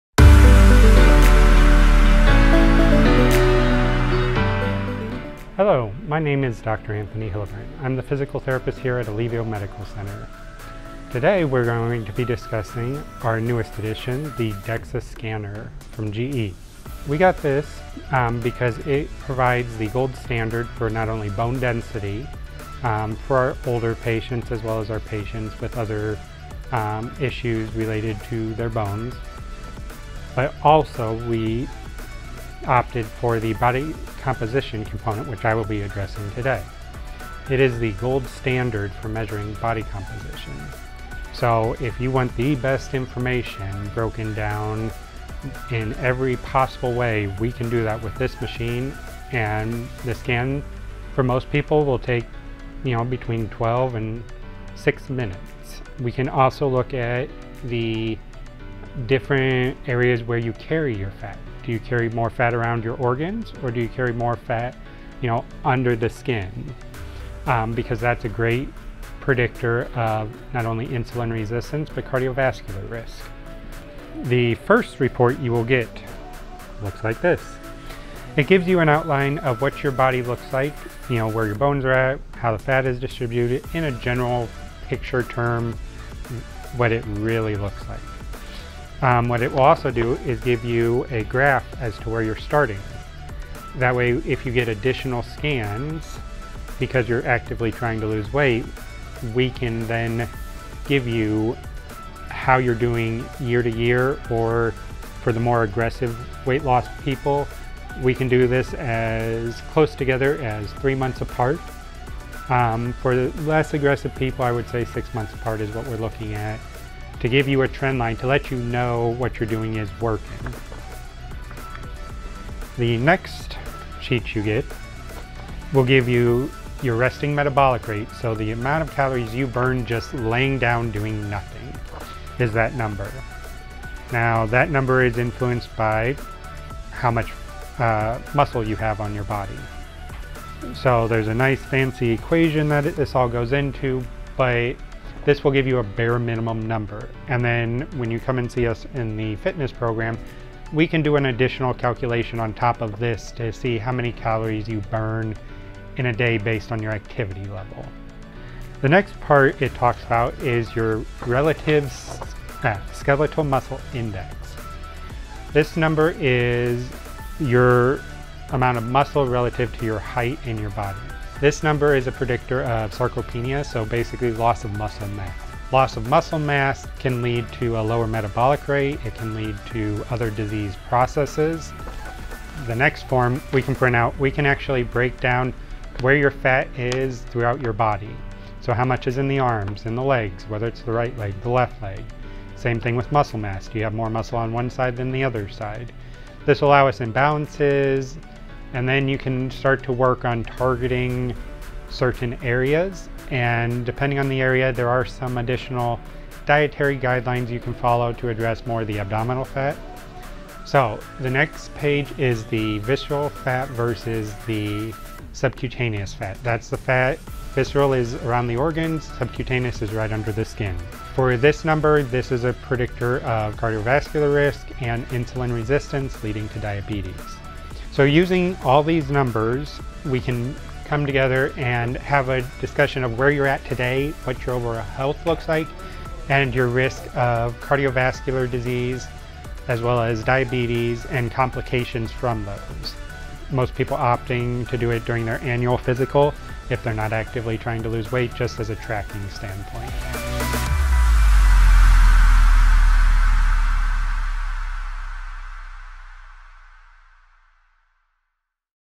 El nuevo escáner en Alivio
Nuestro nuevo escáner DEXA que nos ayuda a determinar la densidad ósea de un paciente pero también la composición del cuerpo para saber dónde se localiza la grasa y la masa muscular. Los datos que nos brinda esta máquina nos ayuda a maximizar esfuerzos de pérdida de peso y también prevenir riesgos cardiovasculares y diabetes entre otros problemas de salud.